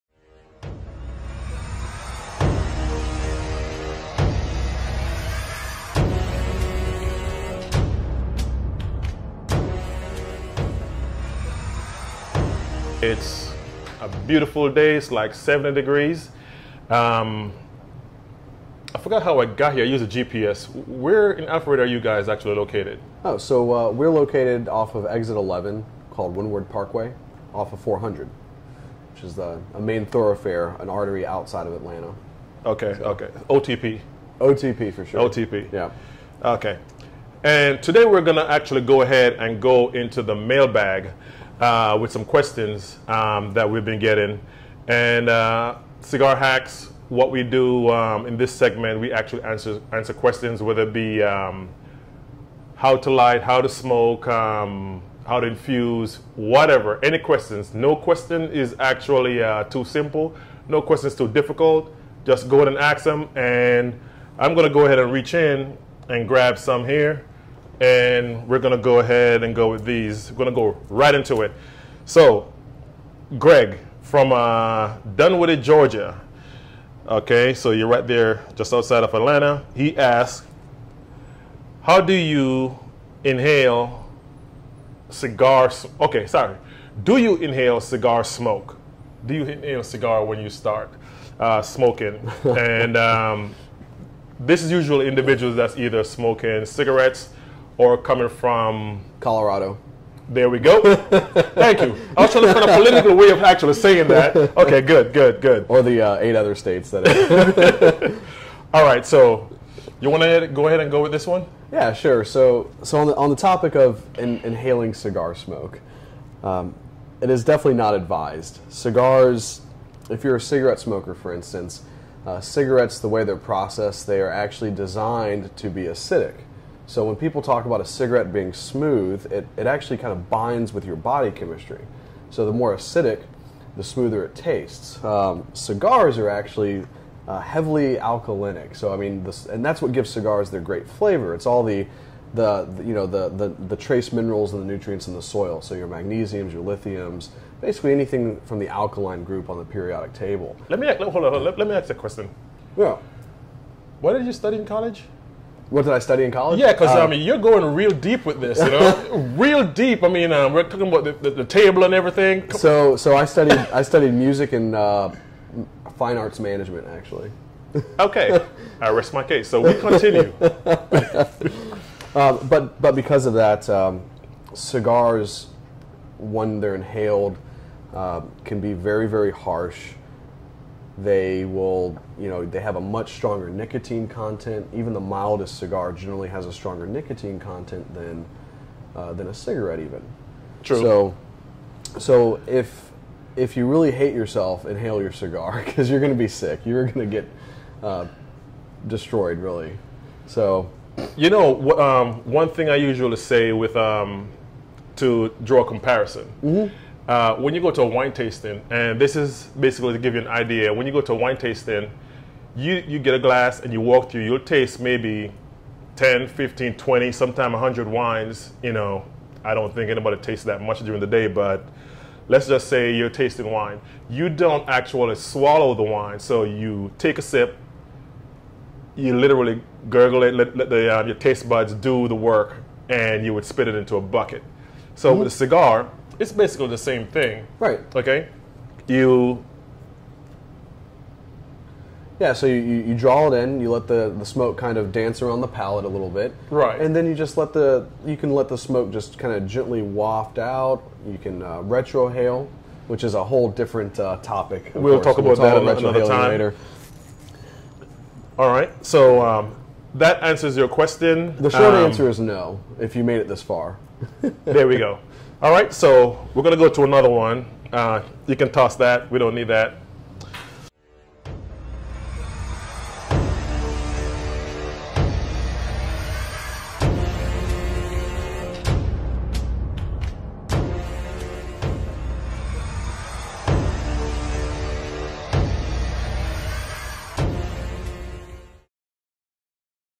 Do you inhale cigar smoke
Do you inhale when you smoke? How does the periodic table play a part in the makeup of the tobacco? How can cigar smoking be similar to wine tasting? Tips on how a professional retrohale and why a novice should not.